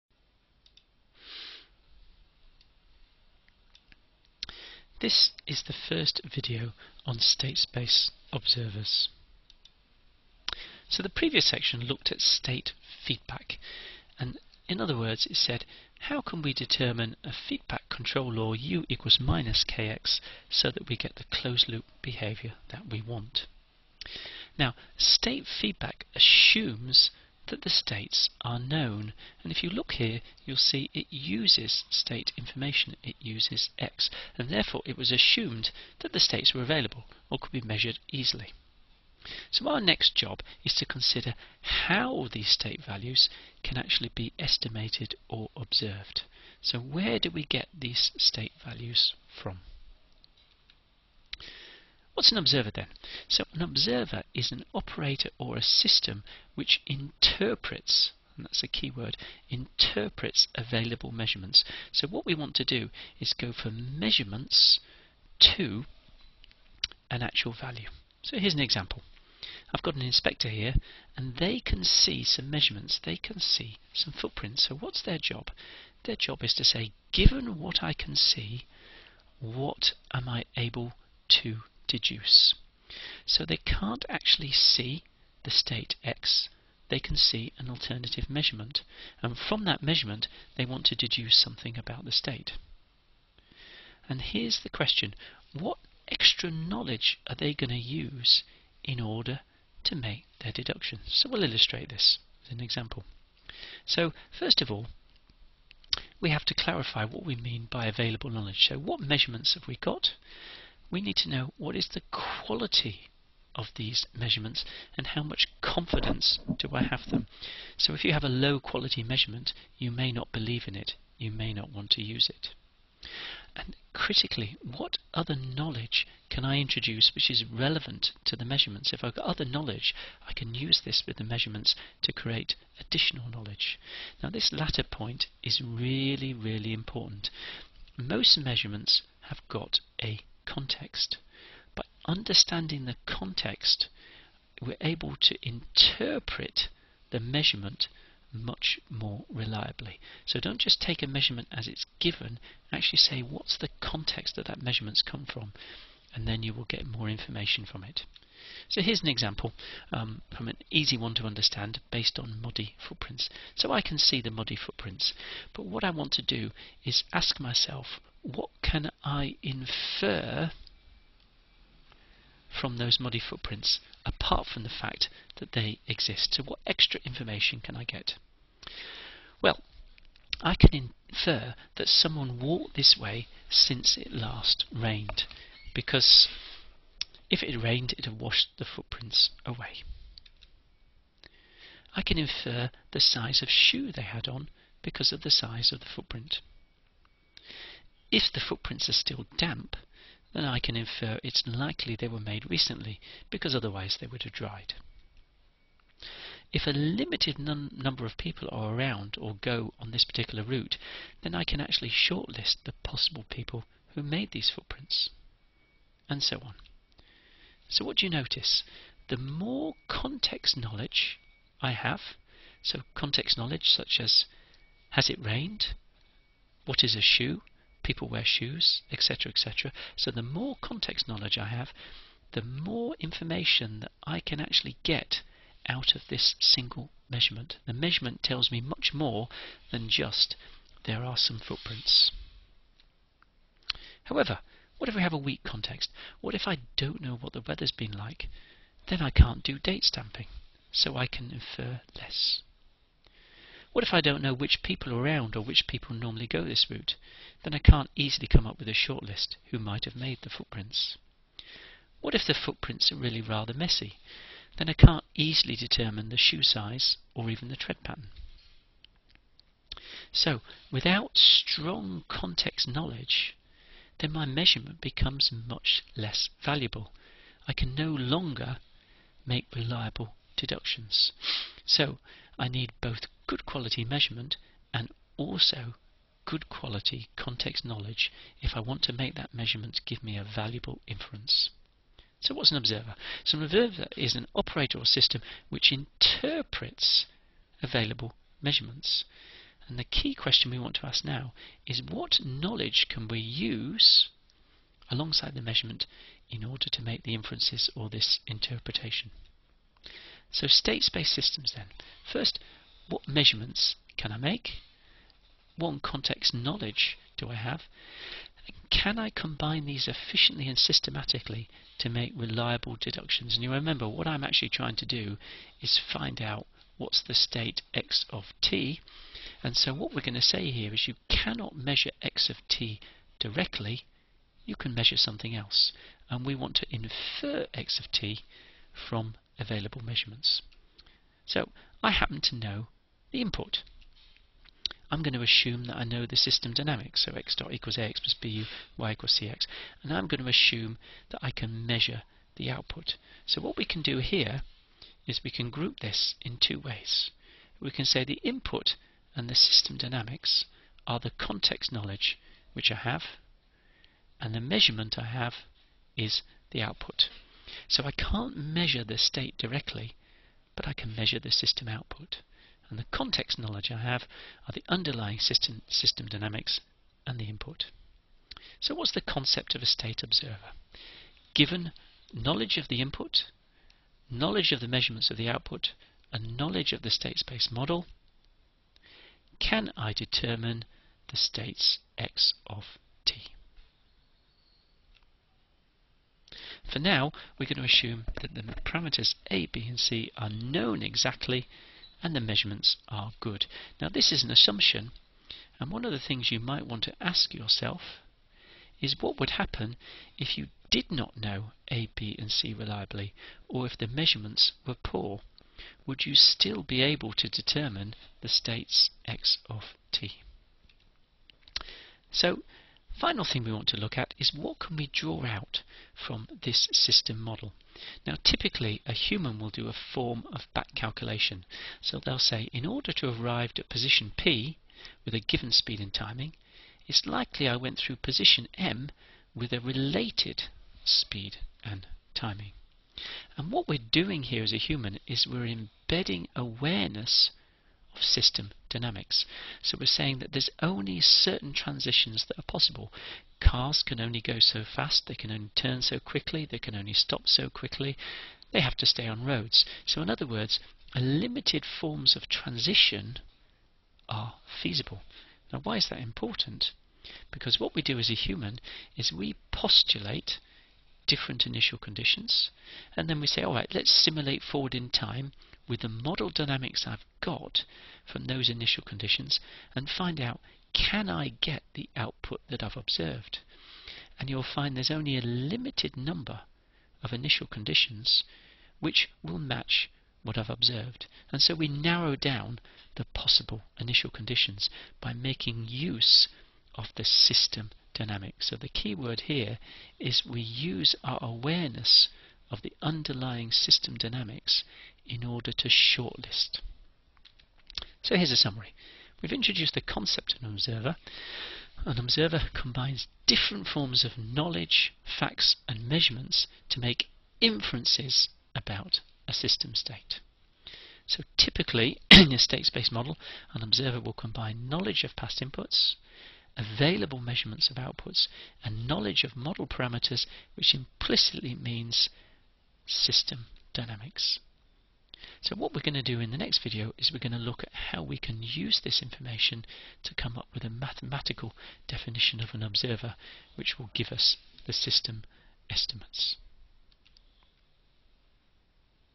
State space observers 1 – introduction
Introduces the concept of an observer using layman's terms and discusses how there is a need to use context information and other knowledge to extract the maximum information from any measurements available.

Lectures aimed at engineering undergraduates. Presentation focuses on understanding key prinicples, processes and problem solving rather than mathematical rigour.